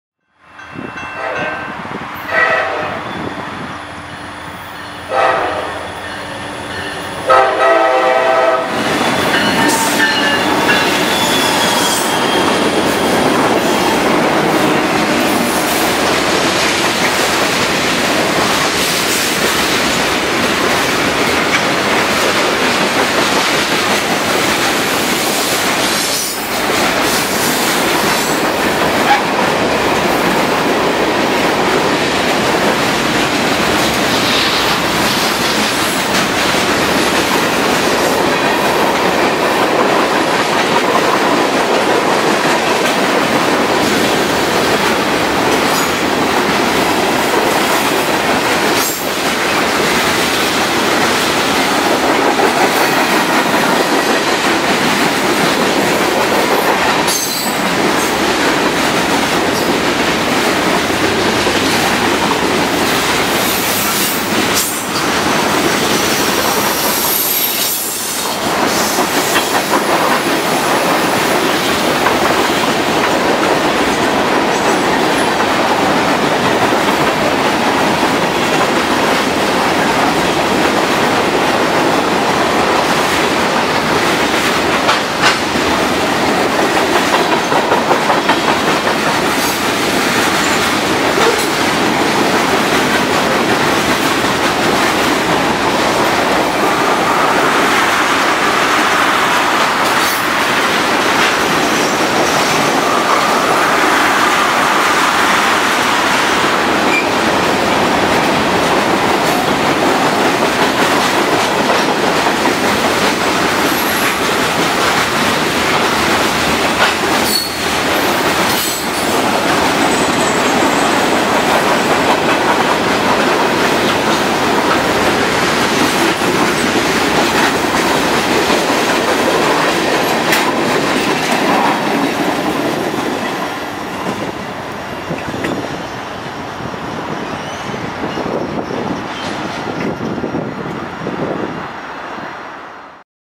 CSX SD40-3 & FURX Geep Lead Q417(9) Through Newburgh
CSX Q417(9) heads south down the River Sub passing Newburgh with a SD40-3 leading 3 other locomotives and a decent sized train. Also notable in this train is a Kasgro 300 Ton - 30’ Depressed Deck Flat Car.Consist is as follows:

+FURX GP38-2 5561
-Manifest
-KRL 300301
-Manifest

(Taken 5/9/15